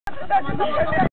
FAM COMPAS I
primer aniversario de en nuevo casas grandes 13, 14 y 15 de febrero del 2009